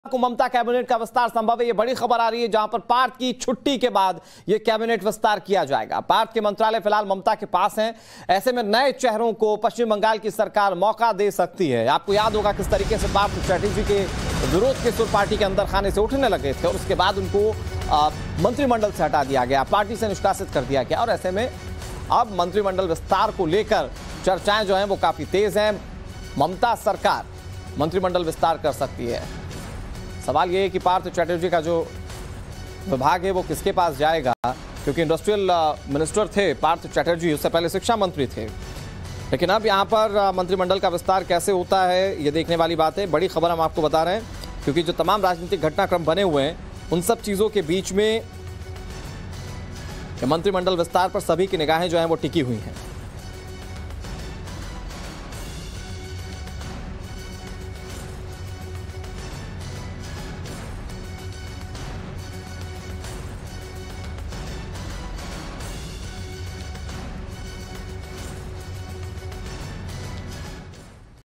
Monday को Mamata Banerjee के Cabinet का विस्तार संभव, नए चेहरों को मिल सकता है मौका | Hindi News
West Bengal में Mamata Banerjee कैबिनेट में फेरबदल हो सकता है। उम्मीद है कि नए चेहरों को मिल सकता है मौका